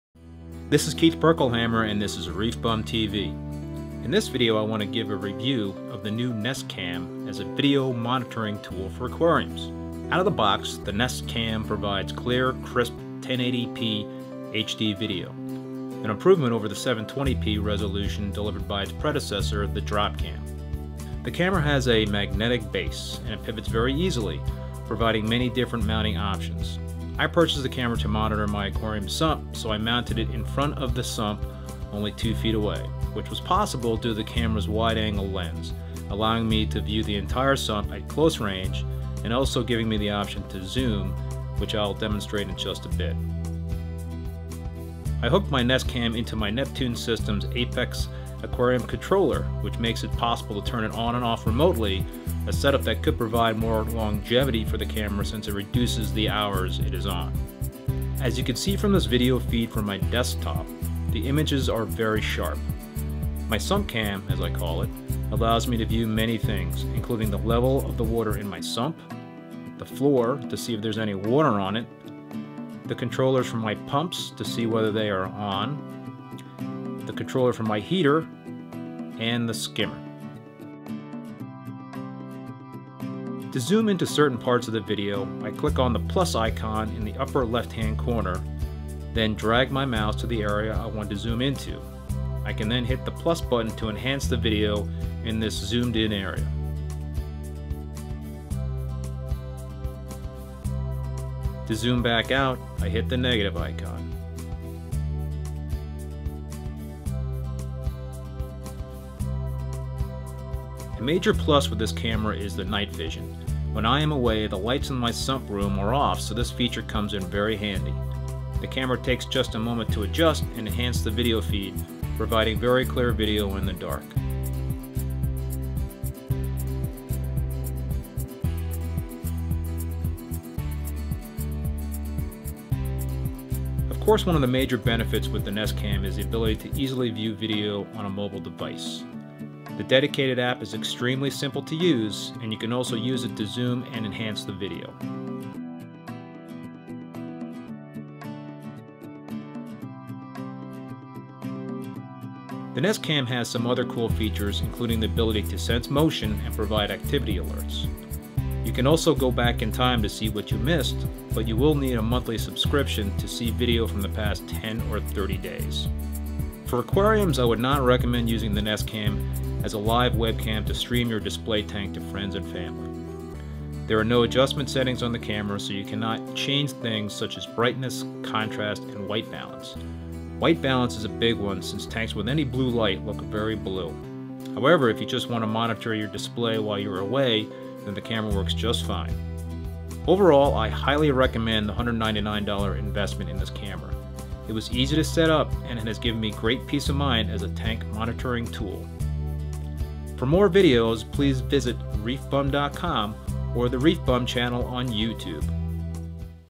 Nest Cam Review
In this video I give my review of the Nest Cam as an aquarium
monitoring tool.

Nest Cam

Canon EF 100mm f/2.8L IS USM Macro Lens

Canon EF 24-70mm f/4.0L